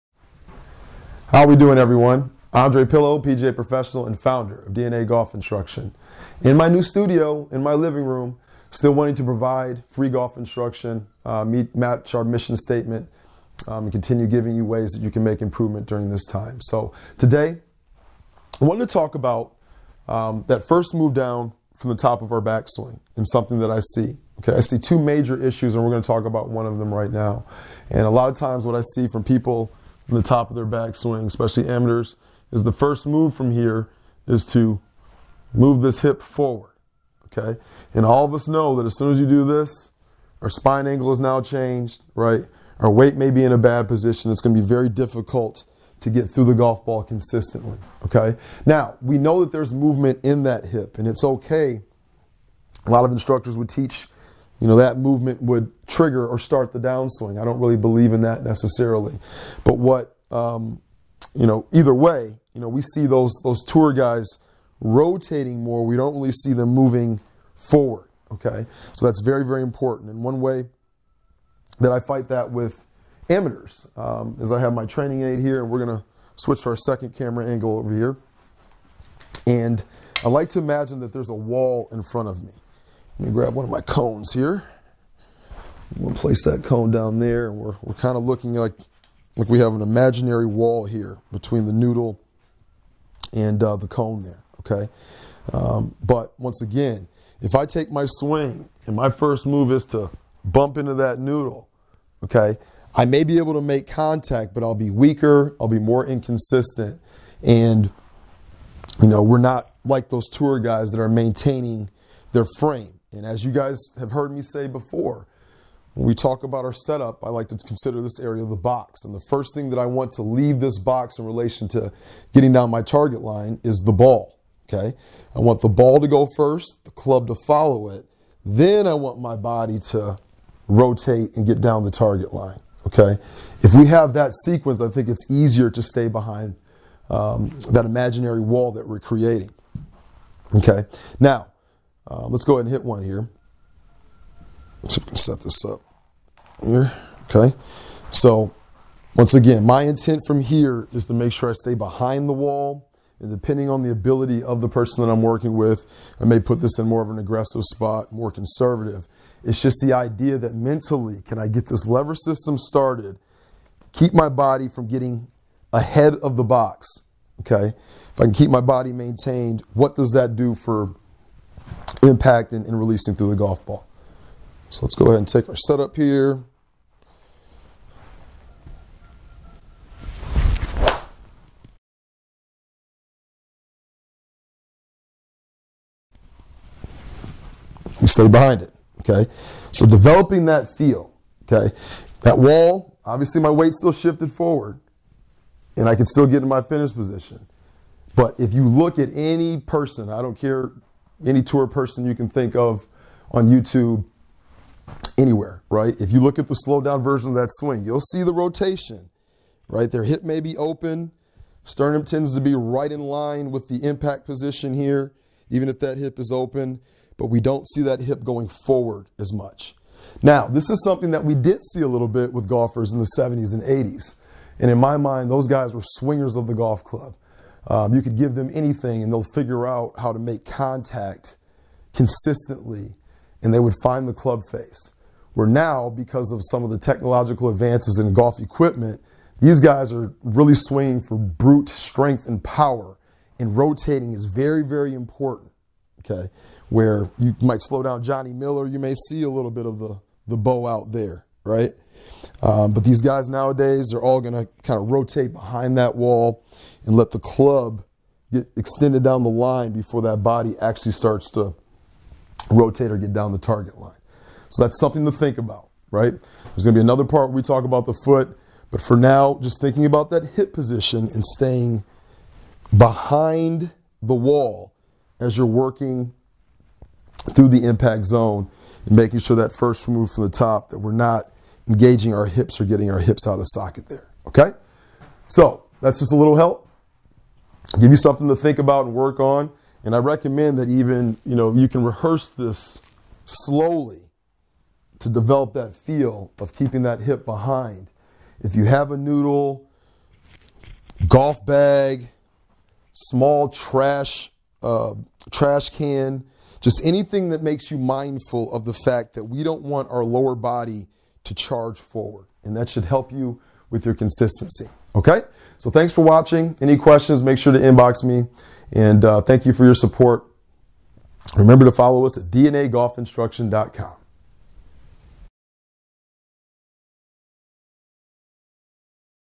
Don't Slide Your Hips!
Help with your downswing and impact... Don't slide those hips...